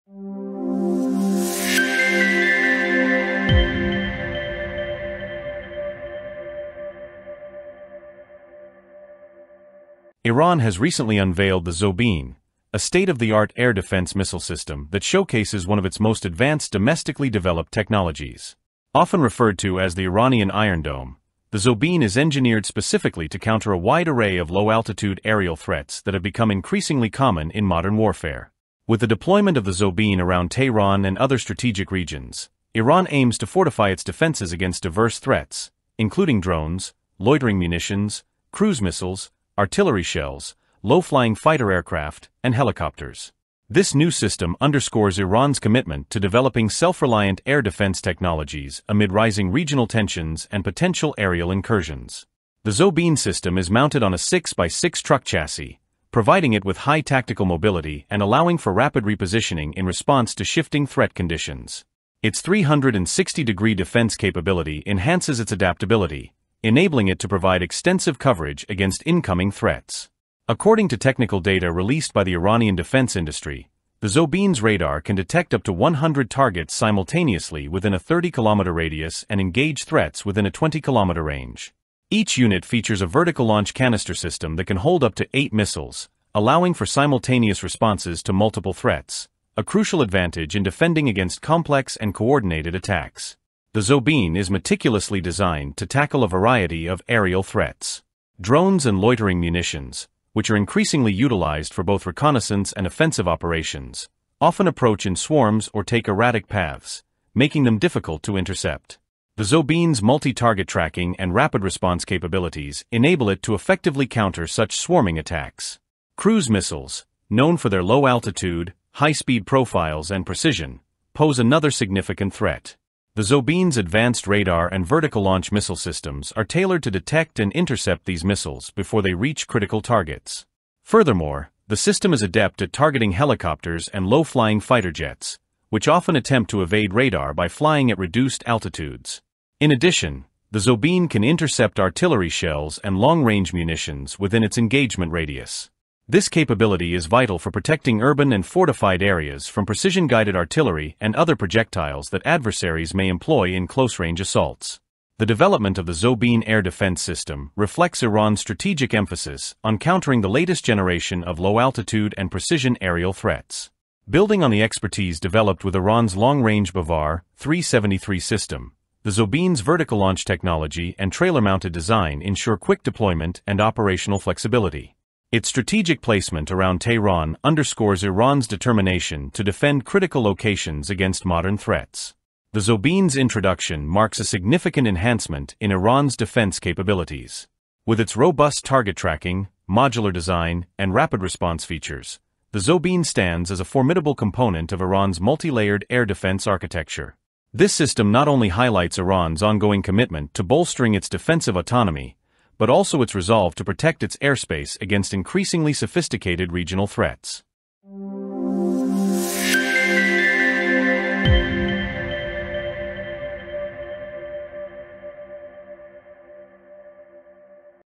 Iran Inroduces Zoubin air defense missile system with 360° interception similar to Israel's Iron Dom
The Zoubin air defense missile system, often dubbed the "Iranian Iron Dome," is one of Iran’s most advanced domestic technologies designed to counter low-altitude aerial threats. With its 360-degree coverage and rapid-response missile capabilities, the Zoubin protects against drones, loitering munitions, cruise missiles, low-flying fighter jets, helicopters, and even artillery shells. Strategically deployed around Tehran and other vital areas, this mobile, 6x6 truck-mounted defense platform has a powerful radar that tracks 100 targets within a 30 km radius and engages within 20 km, making it crucial in defending against swarming and precision attacks. This latest advancement in Iran’s self-reliant air defense technology highlights a new layer in the nation’s multi-tiered defense strategy, leveraging the Zoubin’s vertical-launch capabilities for enhanced airspace protection and security.